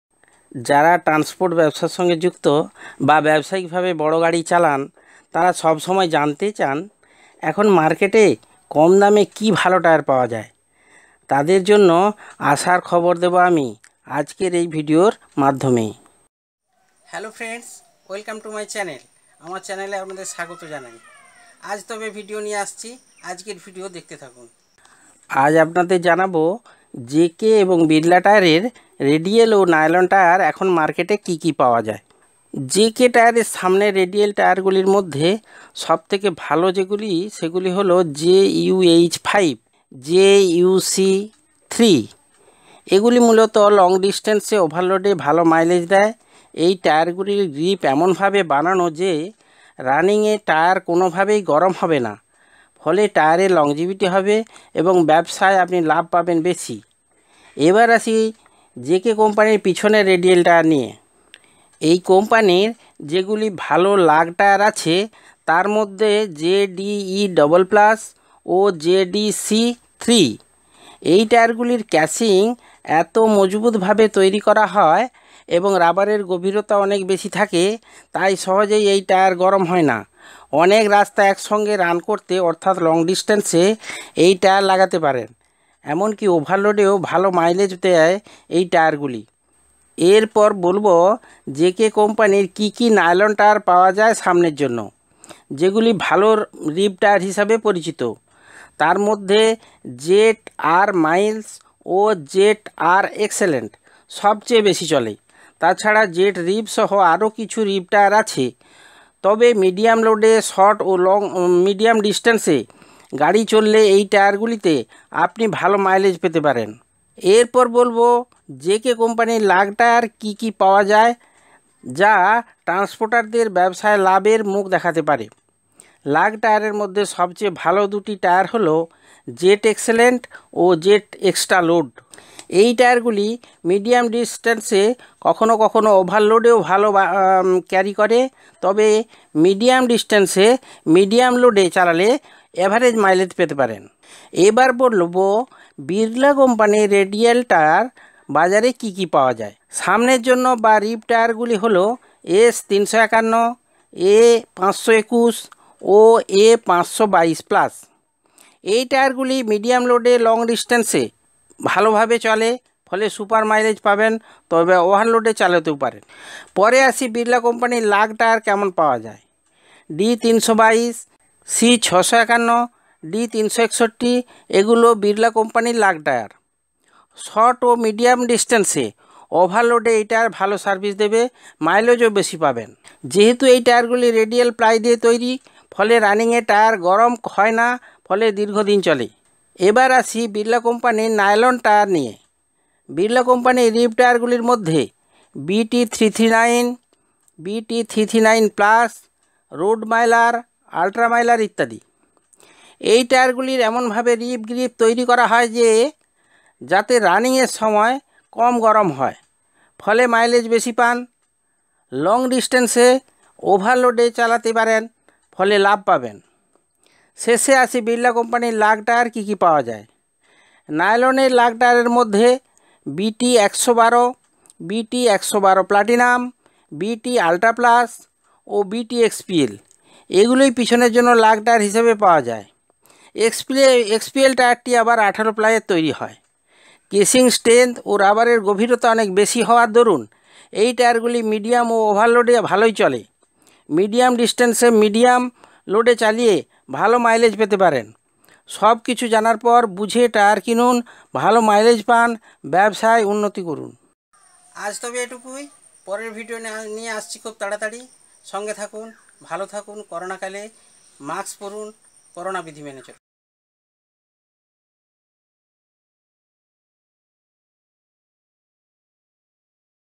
JK vs BIRLA tyres which is better | Car tyre types explained
About this video:
In this video, JK vs BIRLA tyres which is better is well explained in Bengali with presentation in English. Let me know if you have any questions about best bike tyre, tyre of cars, and tyre business in the comment section. Also, free to share any videos or ask any question on the videos of my channel. You can ask your question in English, Bengali and Hindi. Thank you.

Queries Solved:
1. JK radial rib and lug tyres
2. JK nylon rib and lug tyres
3. BIRLA  radial rib and lug tyres
4. BIRLA nylon rib and lug tyres
5. Comparison between JK and BIRLA